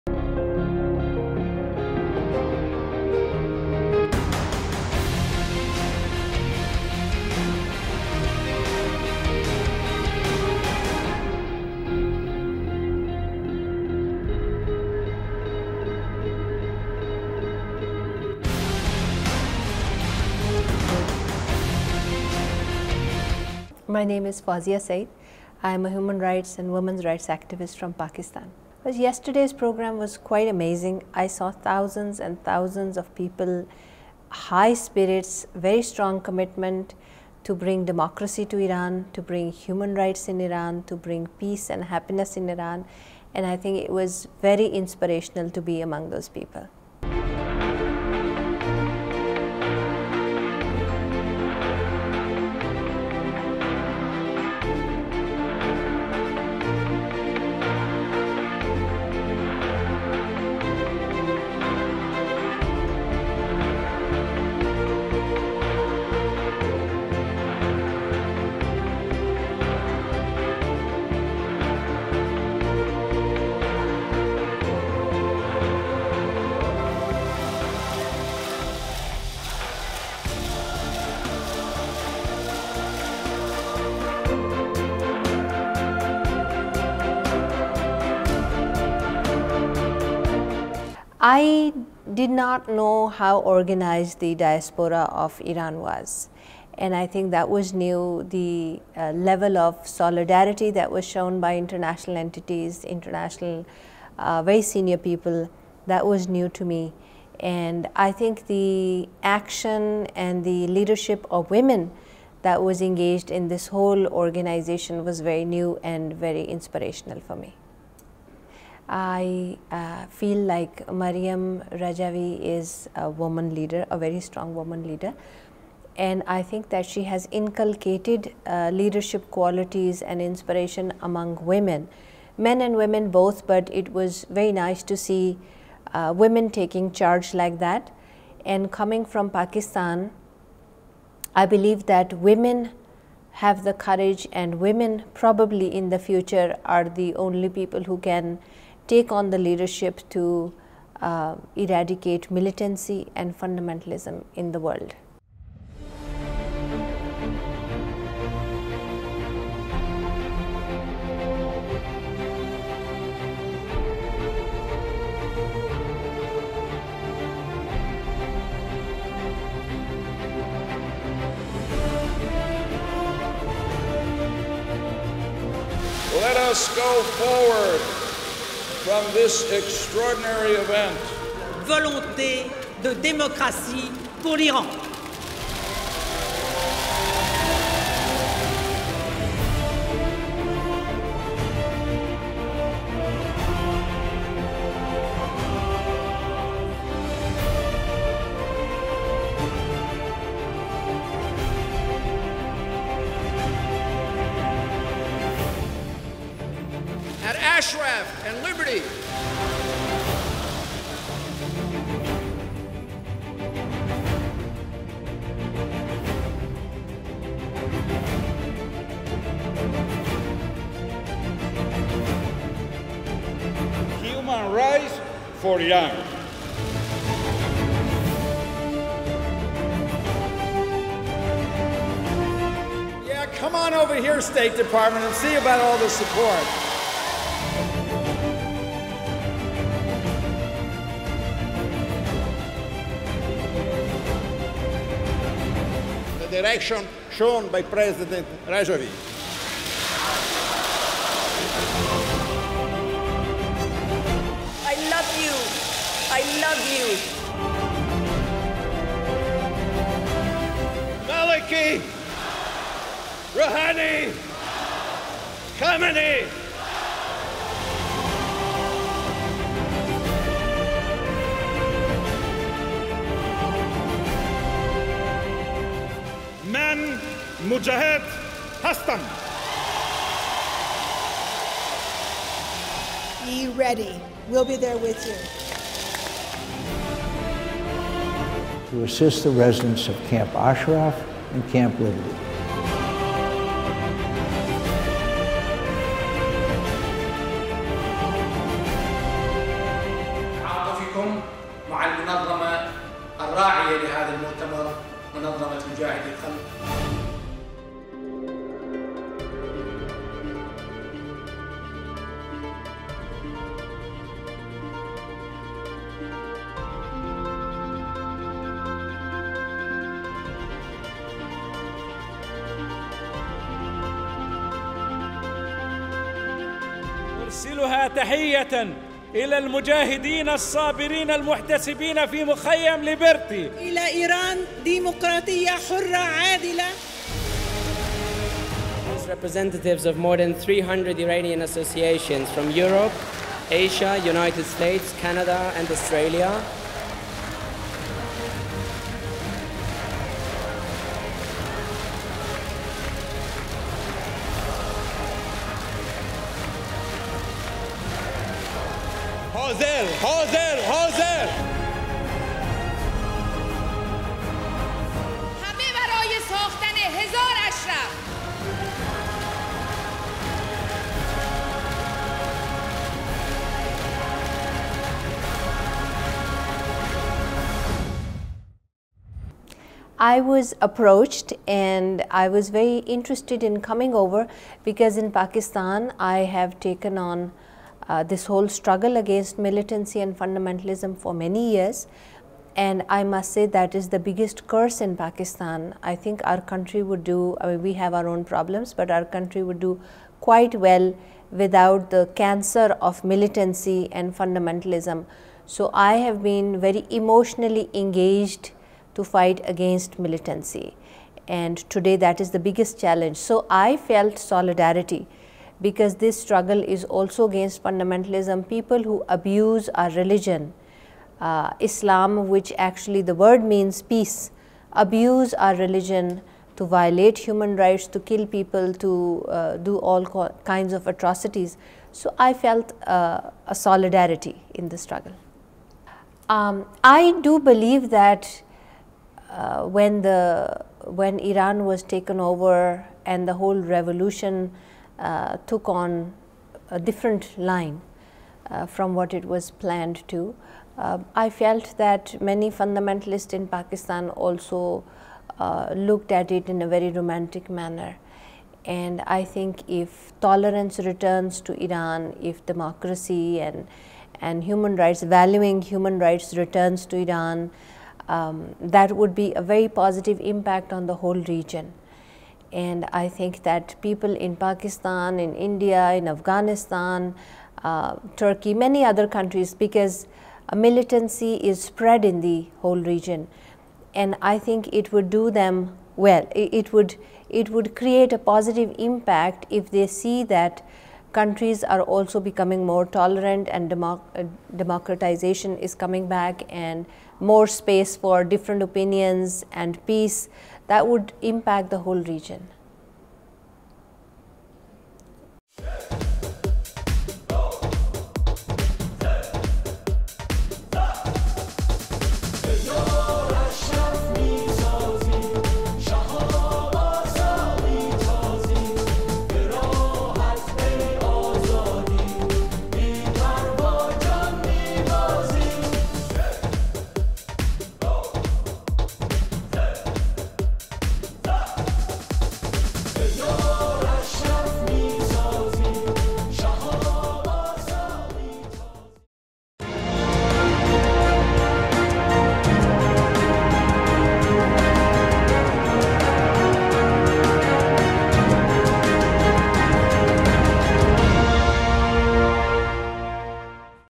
Dr. Fouzia Saeed
Dr. Fouzia Saeed is a well-known women's rights activist from Pakistan and author of many books. She is a social scientist with a PhD from the University of Minnesota.
She participated in the June 27, 2014 grand gathering in Paris, All for freedom for Iran.